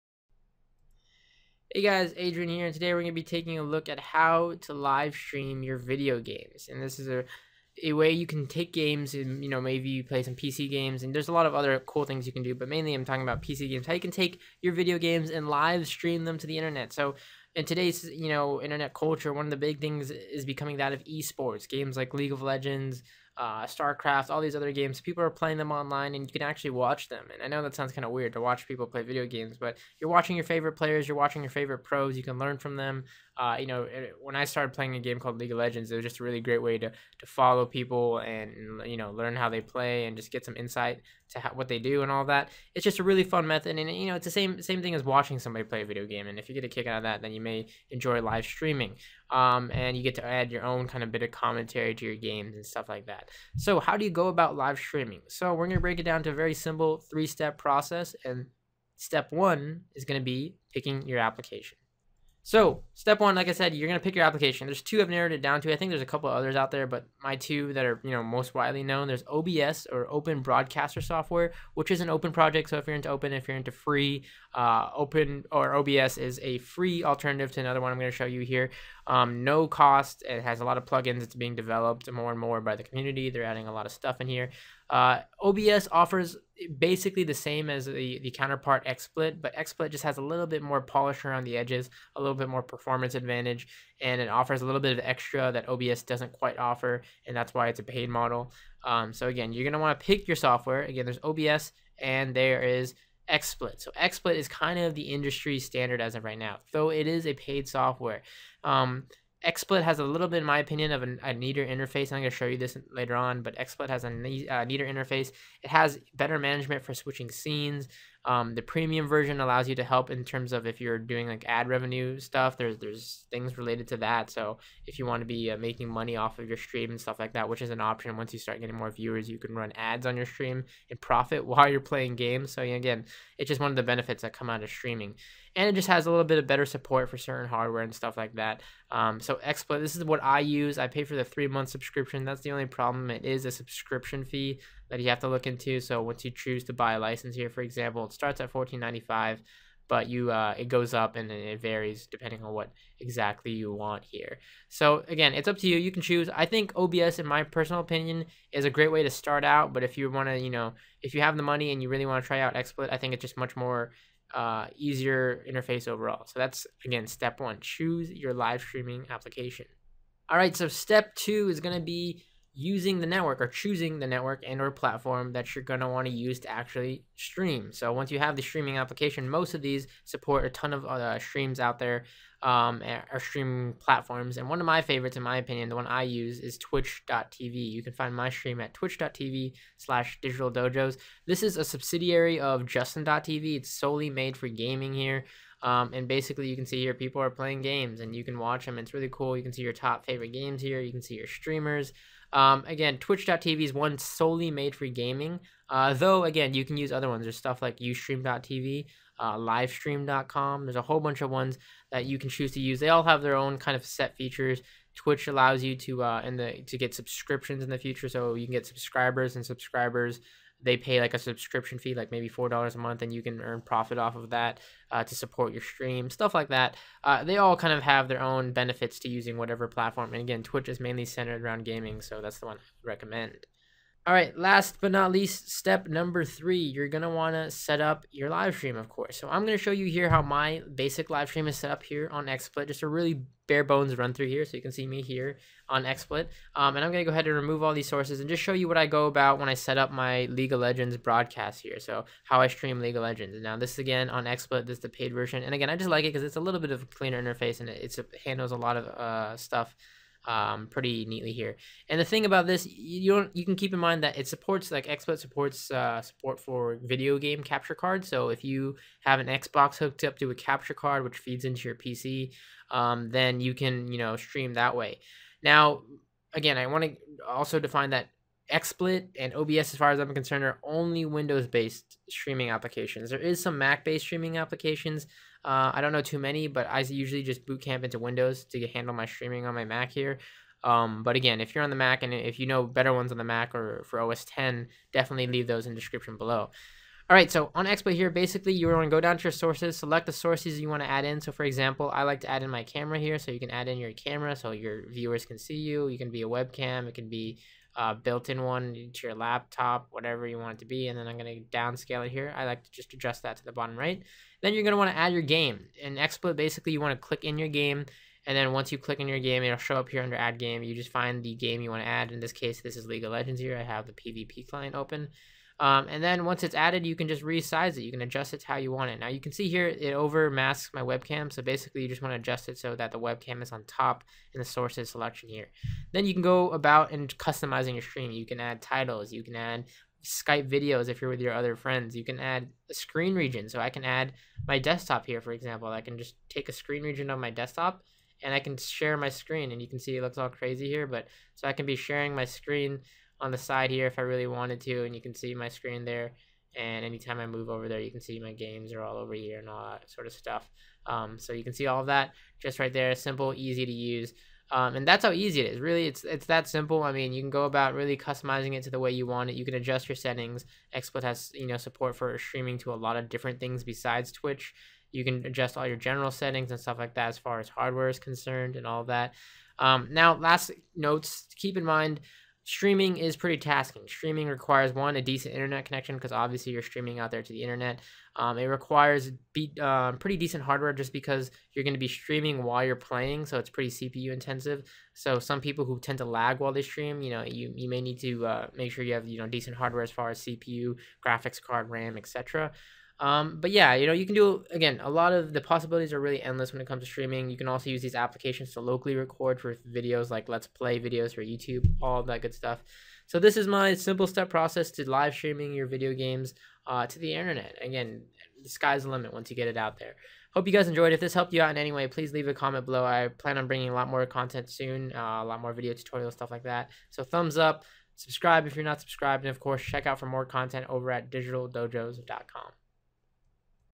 How To: Livestream video games
A look at live streaming video games on the web in a 3 step process.